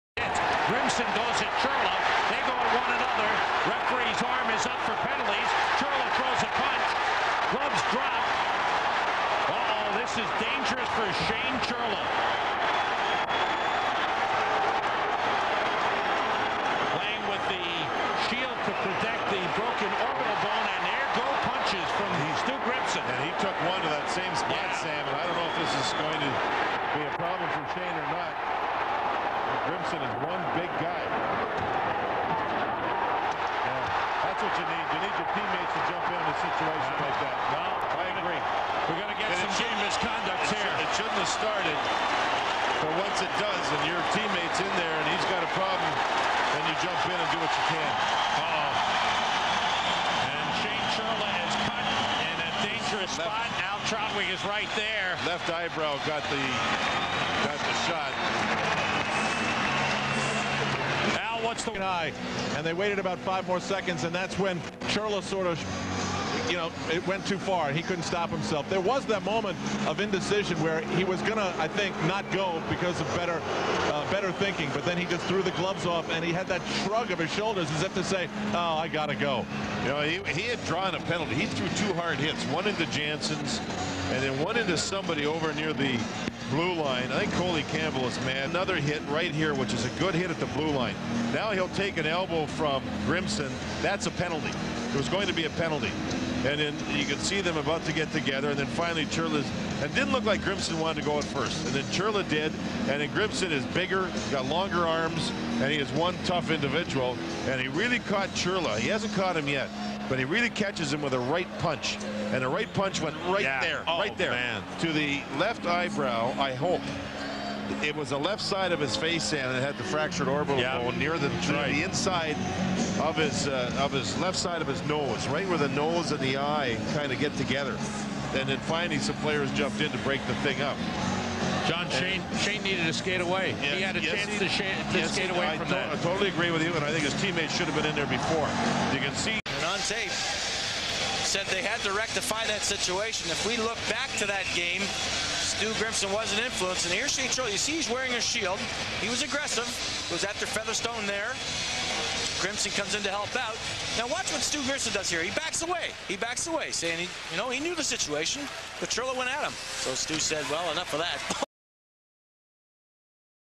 Stu Grimson vs Shane Churla Round 5
Grimson pummels Churla.